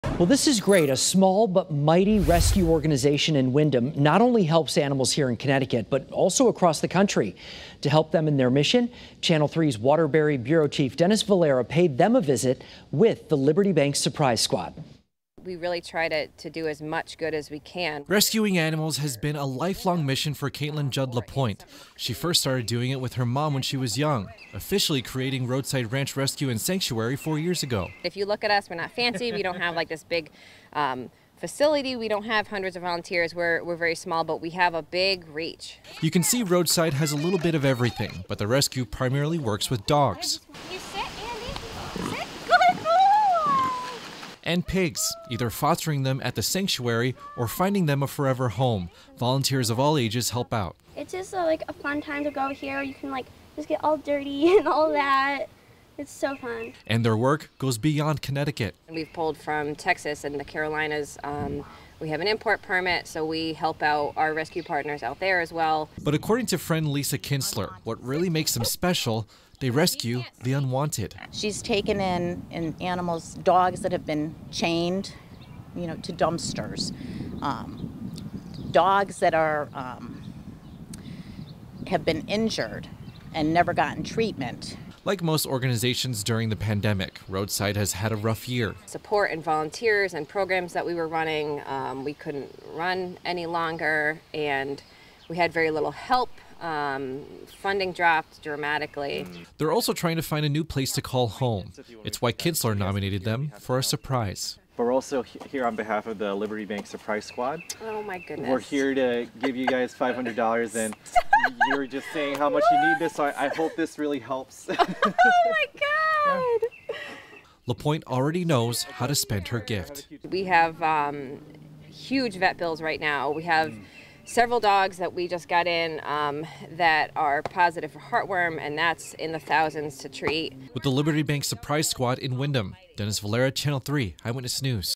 VIDEO: Liberty Bank Surprise Squad visits small but mighty animal rescue in Windham
A small but mighty rescue organization in Windham not only helps animals in Connecticut, but also nationwide.